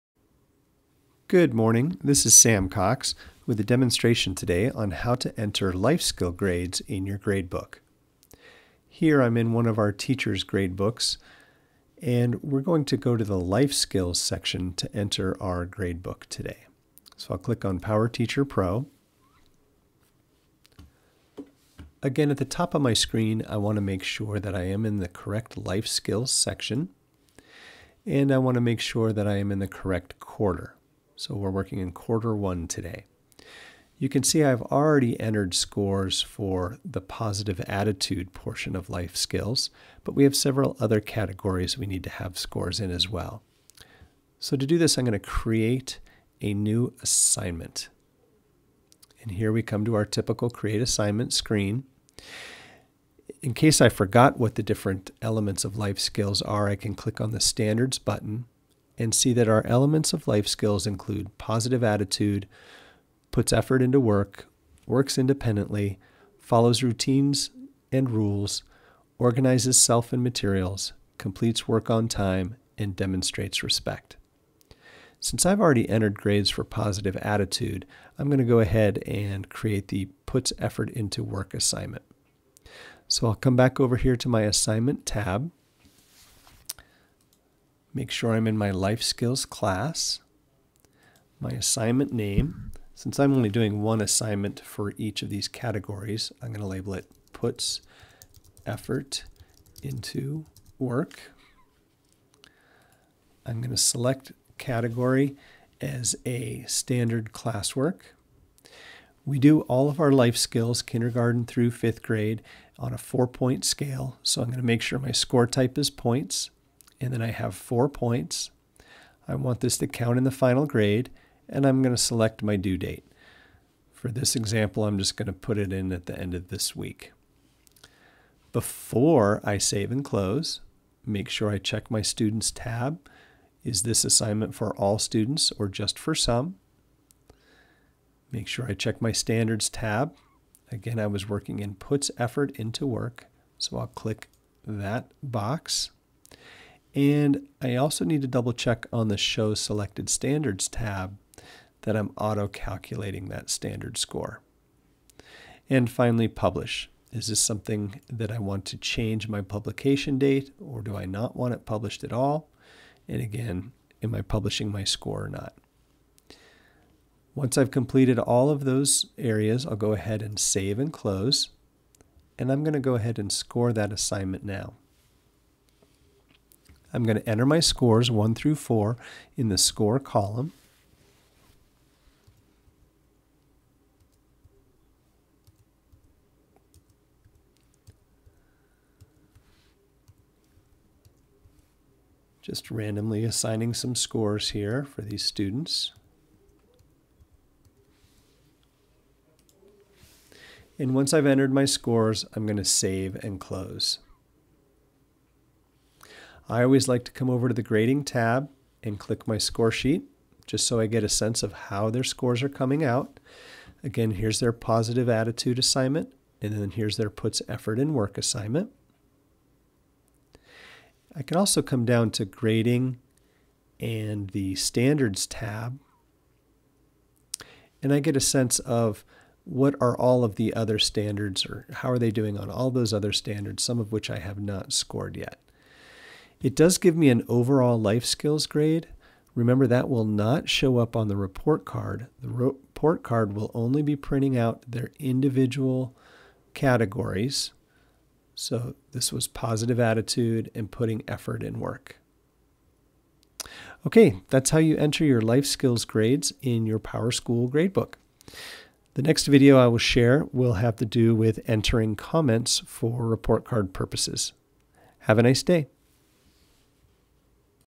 Gradebook - Entering Life Skills Grades
This is a walkthrough on entering Life Skills grades in Powerschool PowerTeacher Pro Gradebook for Delta County School District Teachers.